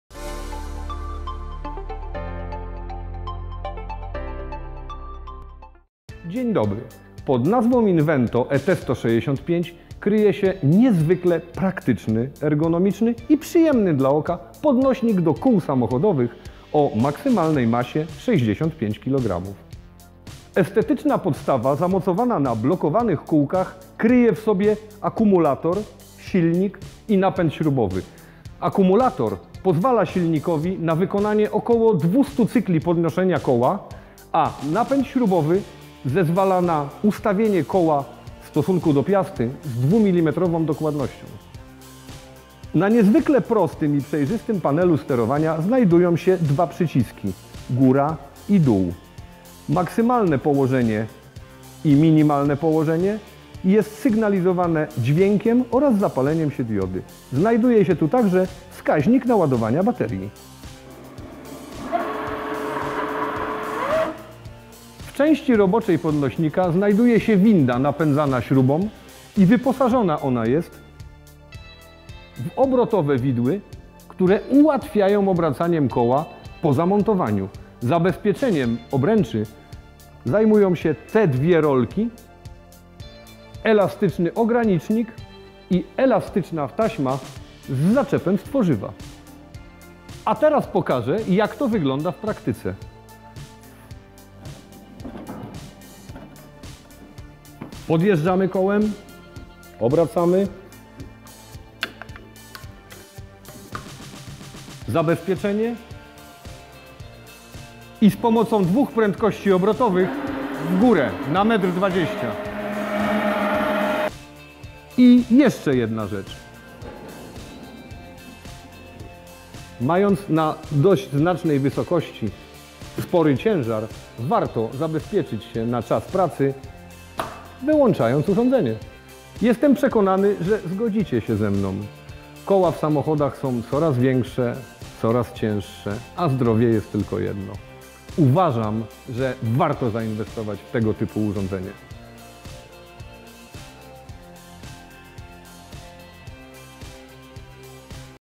Praca z windą do kół INVENTO ET 165
W kolejnym filmie zaprezentujemy Wam jak prosta i przyjemna staje się praca, gdy używacie najnowszej windy do kół marki INVENTO.